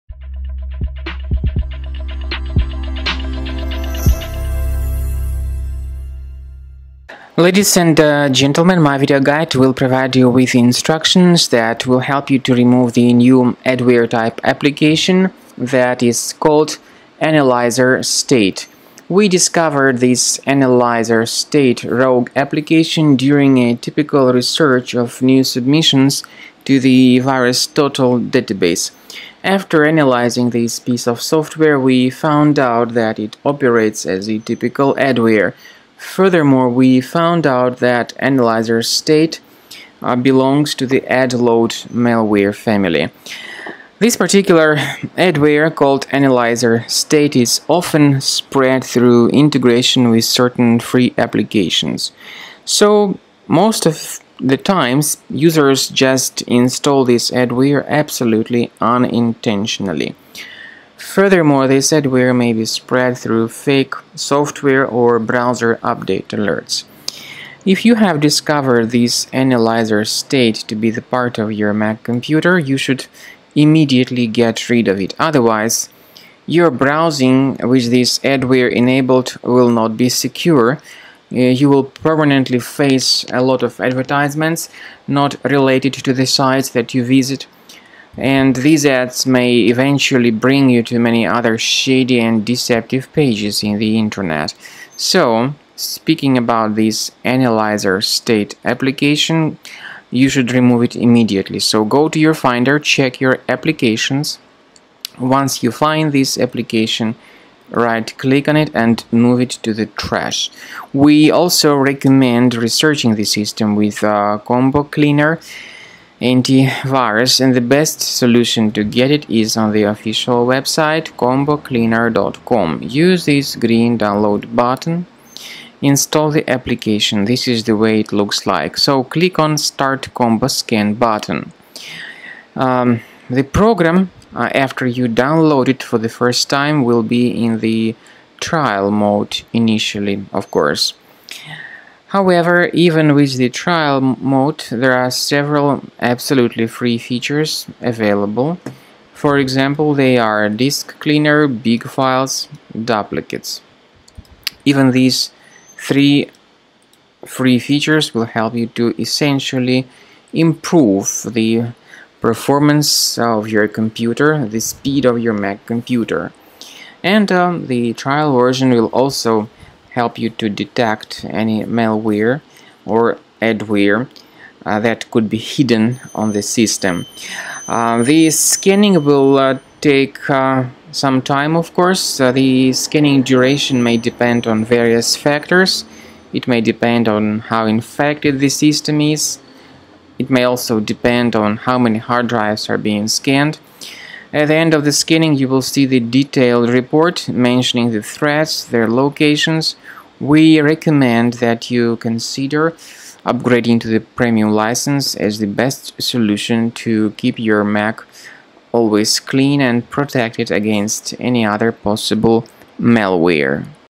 AnalyzerState adware removal (Mac).
AnalyzerState is a new adware-type application designed solely for Mac computers. The adware is from the AdLoad family. It specifically attacks Safari browsers, based on our analysis. The adware is spread through fake software or browser update alerts, so in most of the cases users download and install it unintentionally. Remove the AnalyzerState adware with Combo Cleaner Antivirus or follow the manual suggestions given in the video tutorial.